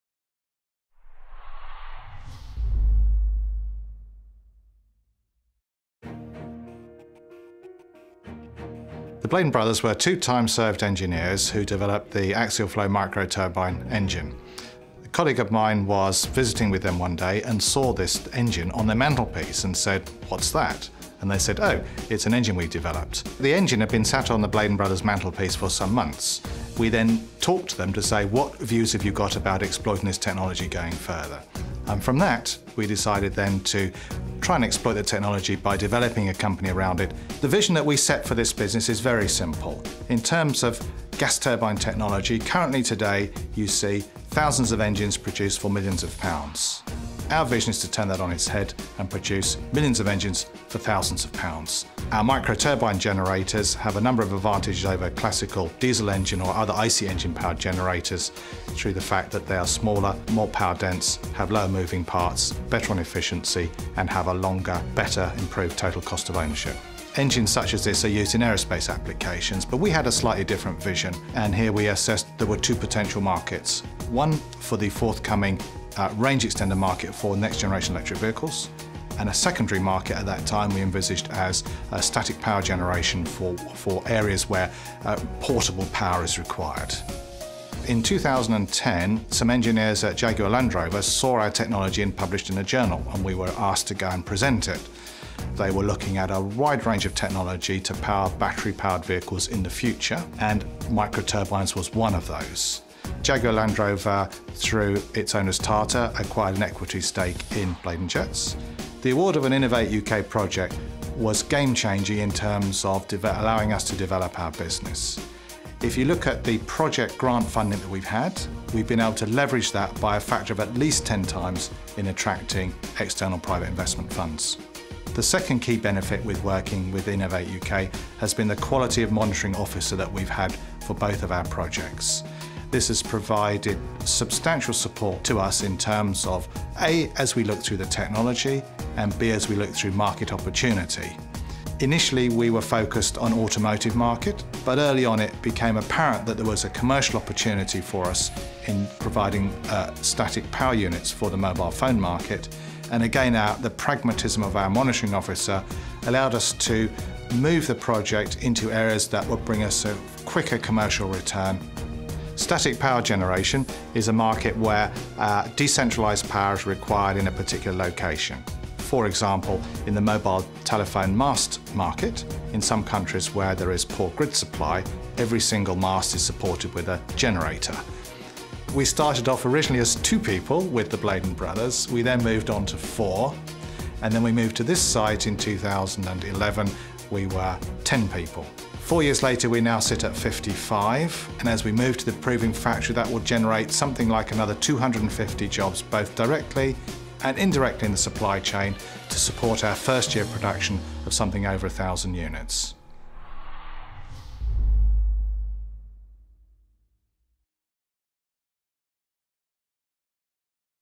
Innovate UK success story: Bladon Jets innovative mobile and low-cost generator takes off
Coventry-based Bladon Jets is about to start manufacturing thousands of innovative micro-gas turbine generators destined to power mobile phone masts in developing parts of the world.

The business has grown rapidly with the support of Innovate UK and is also working with motor manufacturers on developing its engine as a range extender for hybrid electric cars.